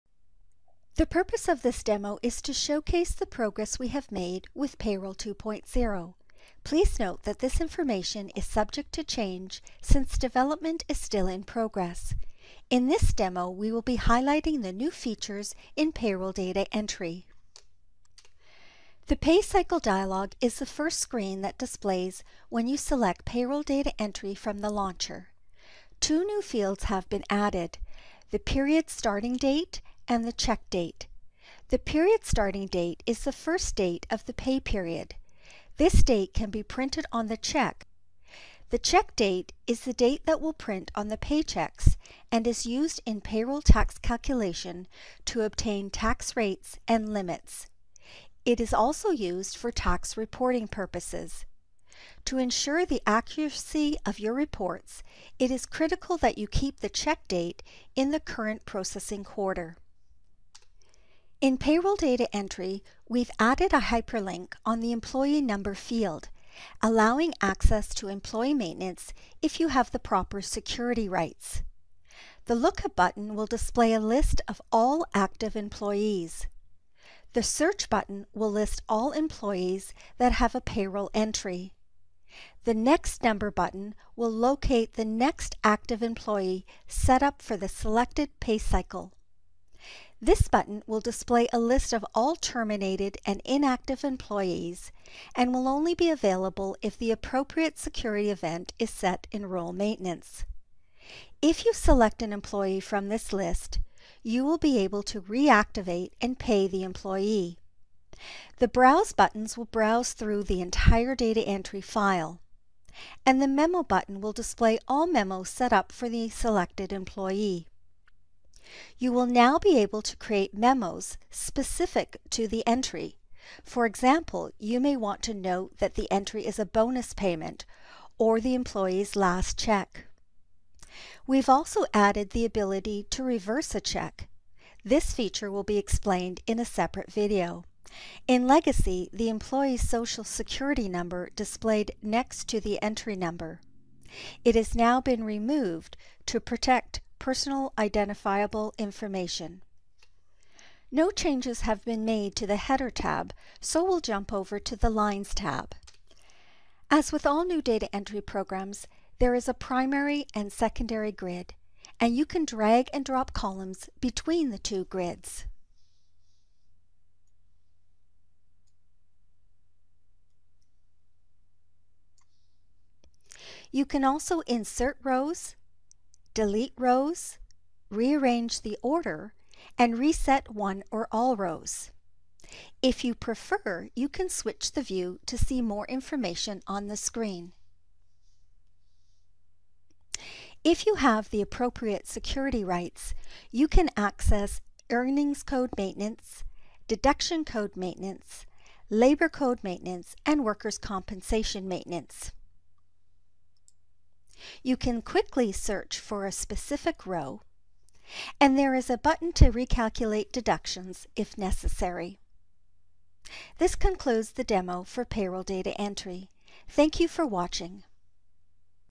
Sage 100 Payroll 2.0 Payroll Data Entry
The latest payroll data entry process has been improved with new options for pay cycle, date controls, check reversals, and more. Watch this video to see how Sage 100 Payroll 2.0 can benefit you.